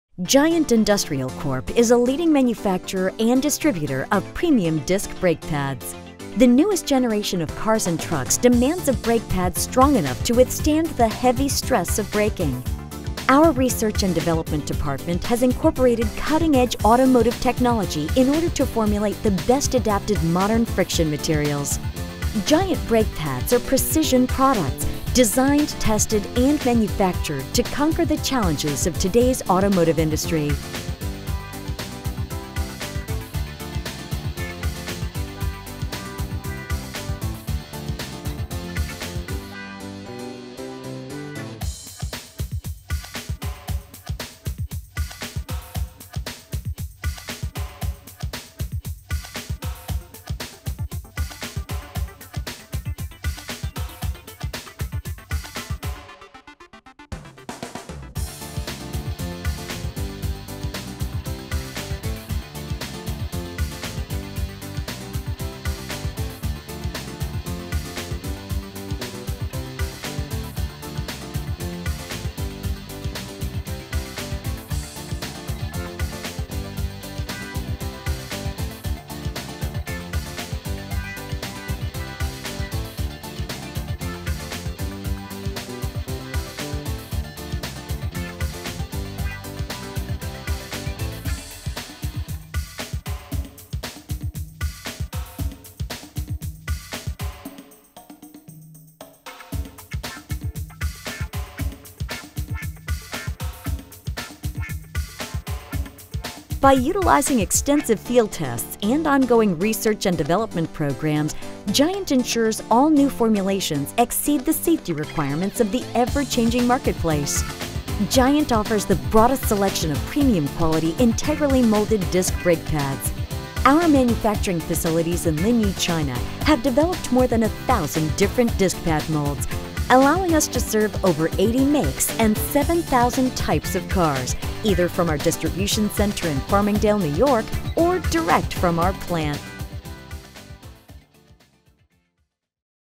Giant Industrial Corp/Giant Manufacturing Inc.
Giant Industrial Corp. will deliver a high quality of service and exceptionalproducts at competitive rates. Our goal is to partner up withour clients and help them generate growth in the marketplace and more importantly growth in revenues.
Giant offers the broadest selection of premium quality integrally molded metallic and ceramic non asbestos disc brake pads in the automotive aftermarket. Giant has approximately 1250 different molds for disc pads at its disposal, these are good for 60 makes and 5000 types of cars.